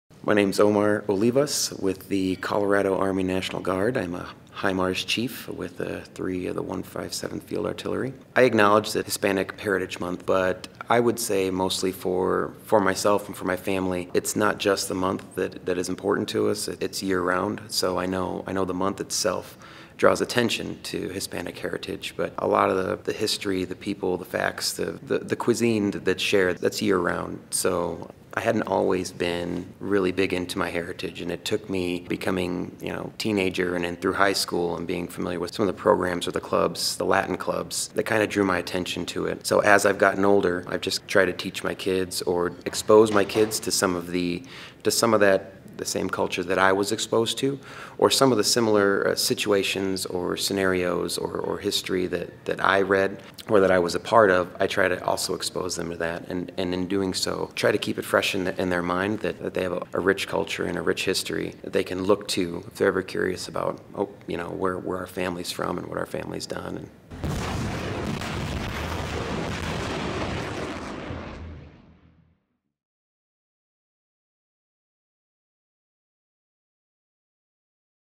Staff Sgt. Oomar Olivas Hispanic Heritage Month 2021
Staff Sgt. Oomar Olivas is a Fire Chief on a HIMARS launcher crew. Here he outlines what his heritage means to him.